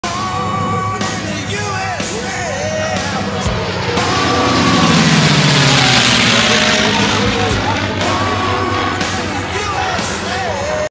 Pair of P-51 Mustangs perform a close flyby!
Pair of P-51 Mustangs flyby during airshow in Stuart, Florida.